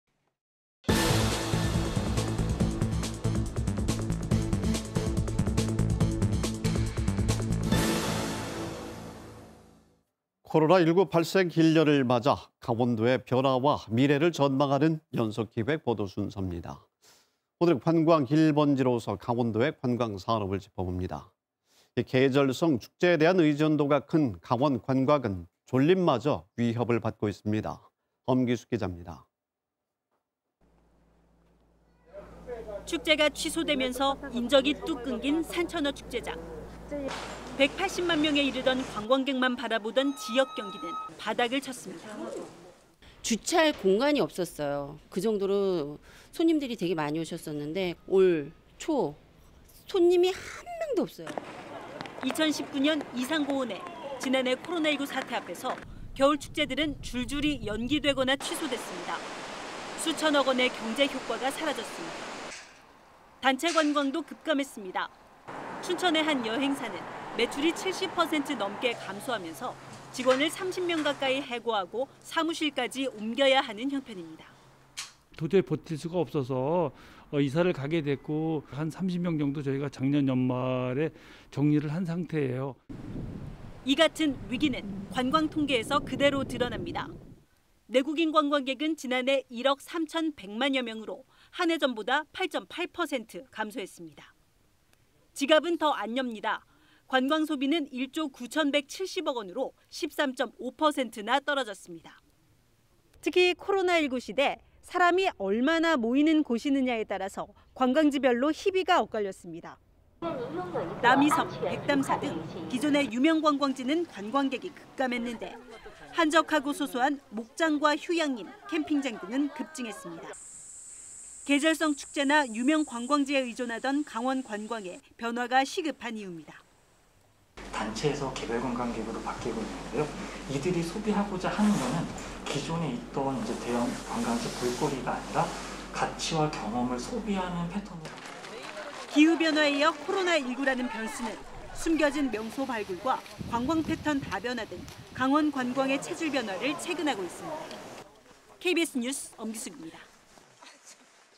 코로나19로 축제 의존 관광 위기…다각화 시급 / KBS 2021.02.24.
[앵커]

 코로나19 발생 1년을 맞아  강원도의 변화와 미래를 전망하는   연속 기획보도 순섭니다.

 오늘은(24일) '관광 1번지'로서 강원도의 관광 산업을 짚어봅니다.

 계절 축제나 유명 관광지에 의존도가 높은 강원 관광은 어느 때보다 큰 위기를 맞고 있습니다.

 엄기숙 기자입니다.

 [리포트]

 축제가 취소되면서 인적이 뚝 끊긴 산천어 축제장.

 180만 명에 이르던 관광객만 바라보던  지역 경기는 바닥을 쳤습니다.

 [조현숙/지역 상인 : "(축제 기간에는) 주차할 공간이 없었어요. 그 정도로 손님들이 되게 많이 오셨었는데…. 올 초엔 손님이 한 명도 없어요."]

 2019년 이상 고온에   지난해 코로나19 사태 앞에서   겨울축제들은 줄줄이 연기되거나 취소됐습니다.

 수천억 원의 경제효과가 사라졌습니다.

 단체 관광도 급감했습니다.

 춘천의 한 여행사는 매출이 70% 넘게 감소하면서,   직원을 30명 가까이 해고하고,   사무실까지 옮겨야 하는 형편입니다.

 [박한성/여행사 사장 : "도저히 버틸 수가 없어서 이사를 가게 됐고, (직원) 30명 정도를 저희가 작년 연말에 정리를 한 상태에요."]

 이 같은 위기는 관광 통계에서 그대로 드러납니다.

 내국인 관광객은 지난해 1억 3,100만 명으로 한 해 전보다 8.8% 감소했습니다.

 지갑은 더 안 엽니다.

 관광소비는 1조 9,170억 원으로, 13.5% 떨어졌습니다.

 특히, 코로나 시대, 사람이 얼마나 모이는 곳이냐에 따라 관광지별로 희비가 엇갈렸습니다.

 남이섬, 백담사 등  기존 유명 관광지는 관광객이 급감했는데, 한적하고 소소한   목장과 휴양림, 캠핑장 등은 급증했습니다.

 계절성 축제나 유명 관광지에 의존하던 강원 관광에 변화가 시급한 이윱니다.

 [양희원/강원연구원 책임연구원 : "단체에서 개별 관광객으로 바뀌고 있는데요. 이들이 소비하고자 하는 건 기존에 있던 대형 관광지·볼거리가 아니라 가치와 경험을 소비하는 패턴으로."]

 기후변화에 이어, 코로나19라는 변수는   숨겨진 명소 발굴과 관광패턴 다변화 등   강원 관광의 체질 변화를 채근하고 있습니다.

 KBS 뉴스 엄기숙입니다.

 촬영기자:김수용